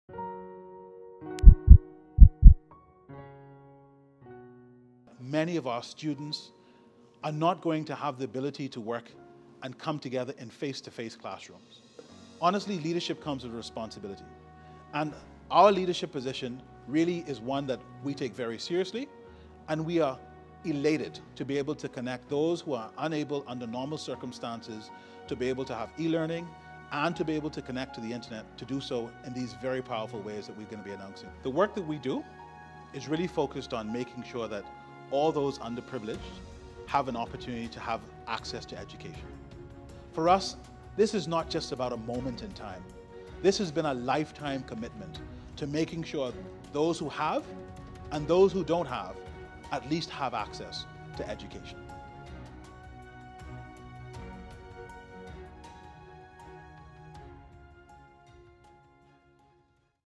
REVNET Lifeline | Leveraging Our Strength
As a champion for digital inclusion, REV is committed to bridging the digital divide for the most vulnerable students across the archipelago. Our aim is to ensure that every child has access to the services they need, to enjoy the quality education they deserve.

Learn more by visiting rev.bs/REVNET-Lifeline today.